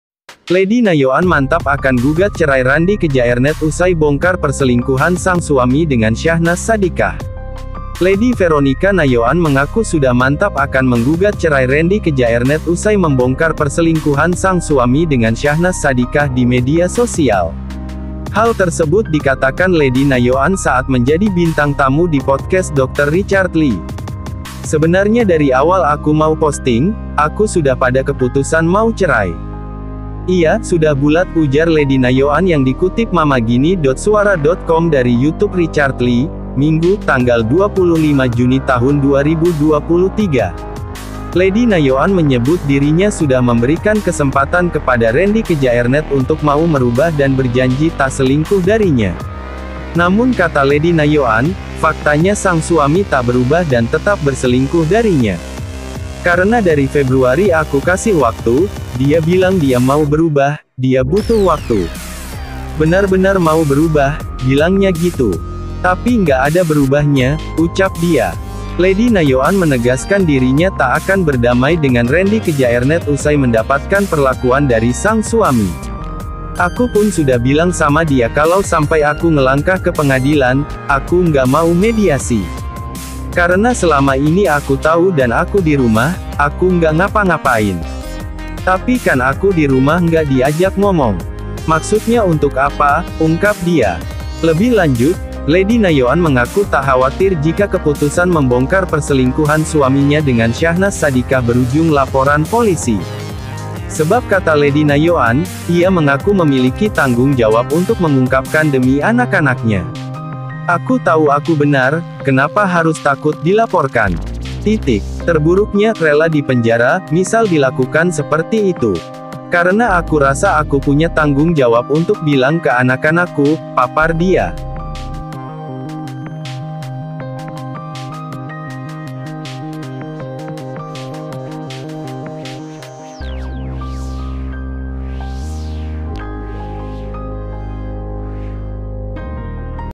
Lady Nayoan Mantap Gugat Cerai Randy Kjaernet Usai Bongkar Perselingkuhan Suami dngn Syahnaz Sadiqah
Lady Nayoan Mantap Akan Gugat Cerai Randy Kjaernet Usai Bongkar Perselingkuhan Sang Suami dengan Syahnaz Sadiqah